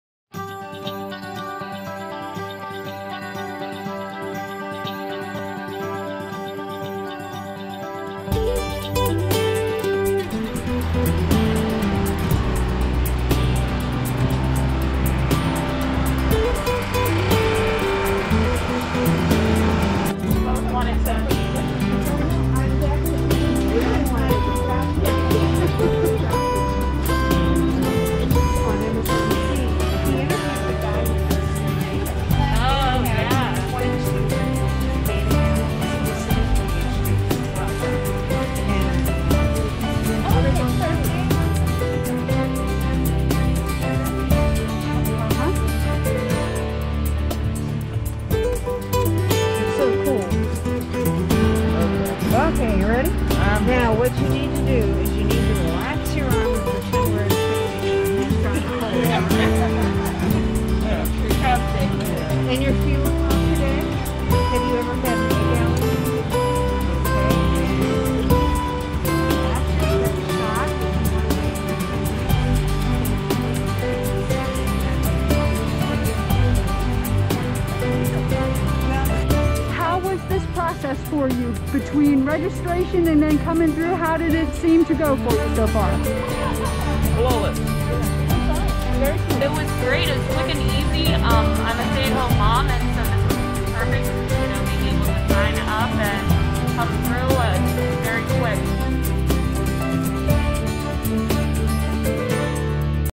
HCHD Seasonal Flu Drive-thru Vaccination Clinic at St. John Baptist Church
HCHD's seasonal influenza vaccination clinic held at St. John Baptist church in Columbia on November 1, 2020.  See how the clinics function behind the scenes.